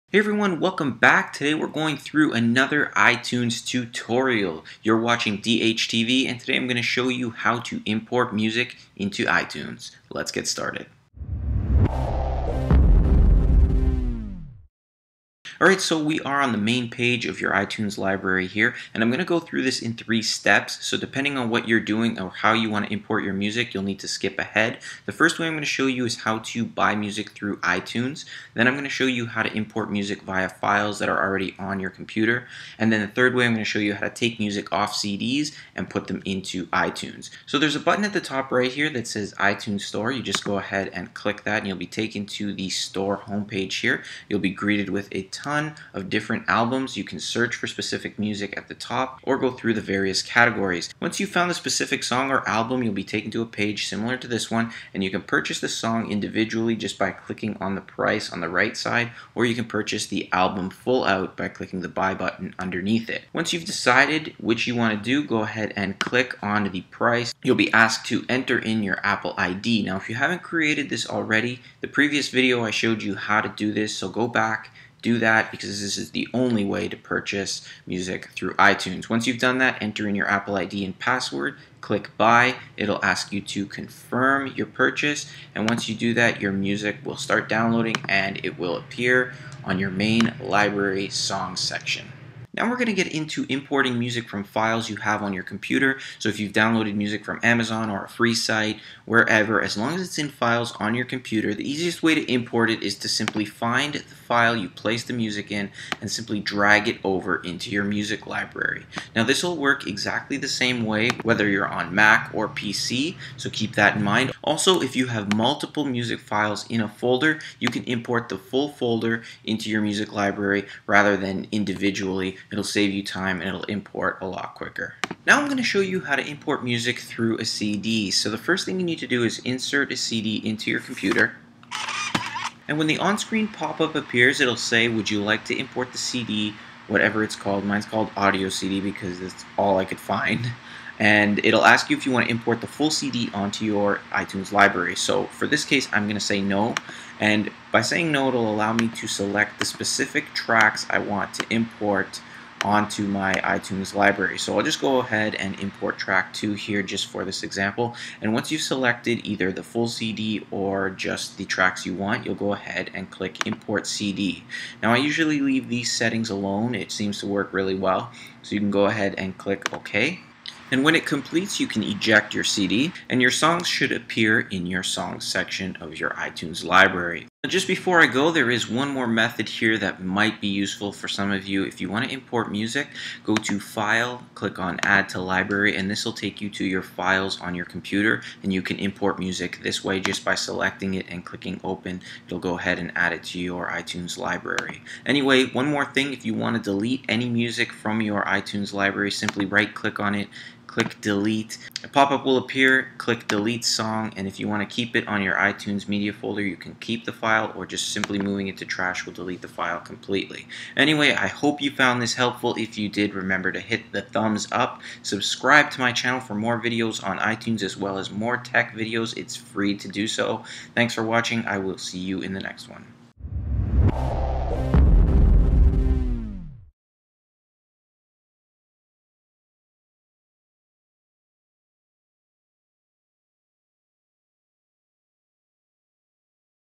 iTunes Tutorial: How To Import and Transfer Music and CD's To iTunes
Showing you how to import and transfer music and CD's to iTunes. Importing music to iTunes is easy if you know the right method. I will show you how to transfer music from files to iTunes, how to purchase music from the iTunes store and import music from your CD's.